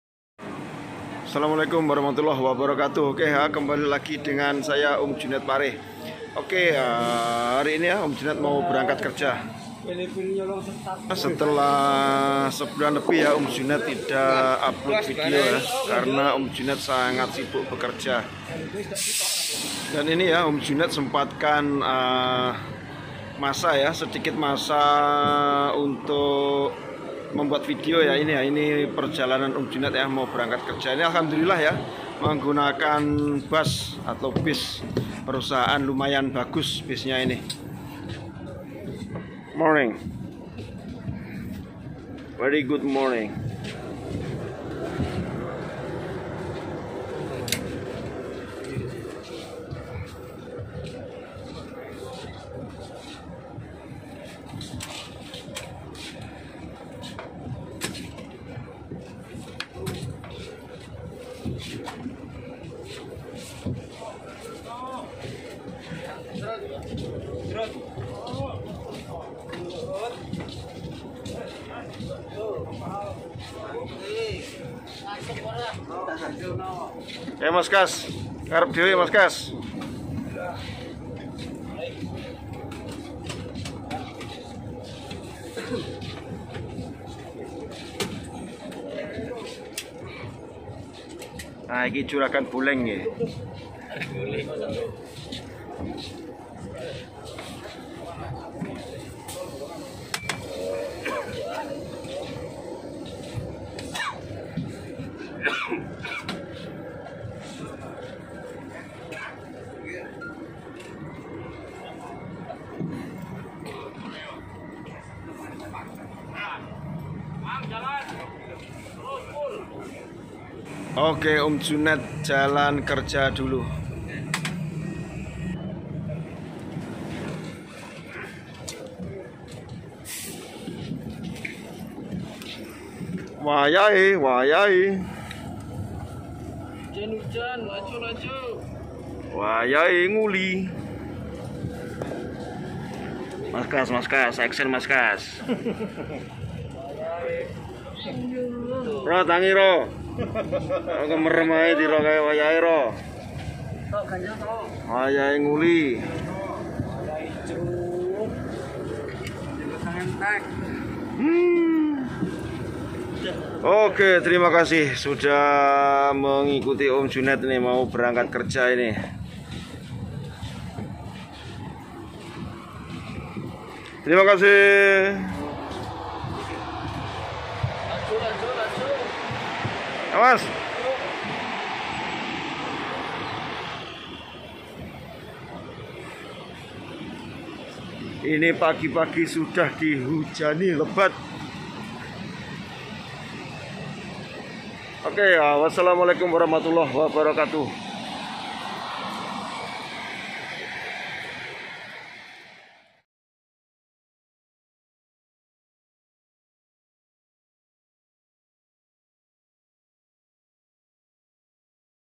PERJALANAN BERANGKAT KERJA TKI BRUNEI MENGGUNAKAN BUS MEWAH || VLOG TKI BRUNEI
PERJALANAN BERANGKAT KERJA TKI BRUNEI,inilah perjalanan berangkat kerja saya kerja di brunei,setiap hari kami menggunakan transportasi yang di sediakan oleh perusahaan ya itu bus yang sangat bagus